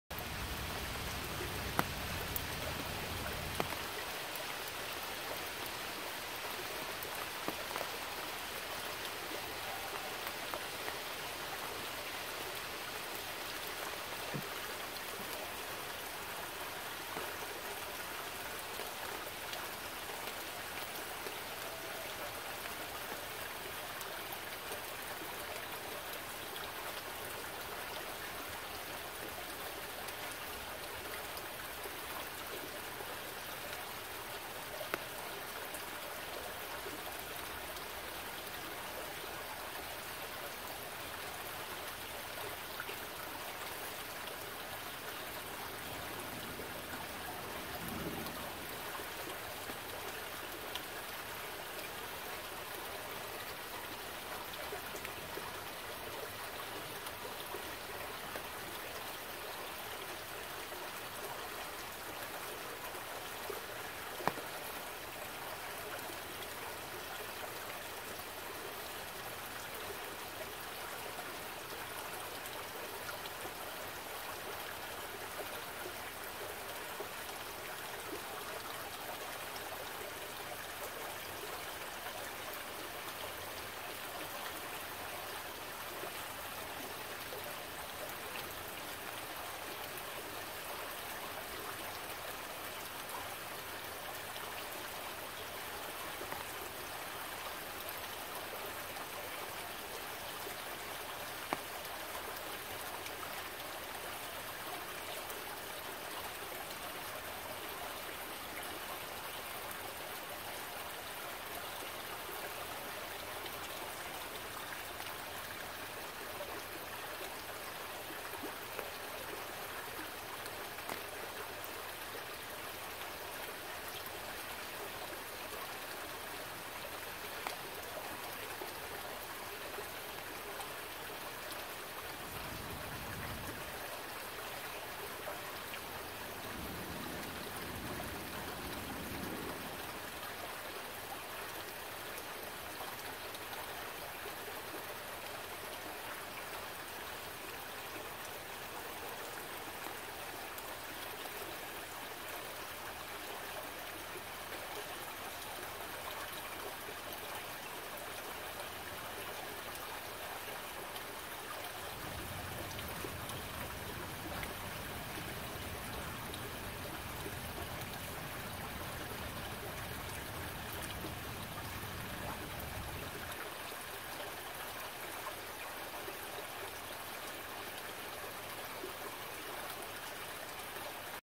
Relaxing rainy day near a stream in a forest.
Relaxing rainy day in a forest I visited today.  Near a stream a rainy view. Visual beauty. Auditory pleasure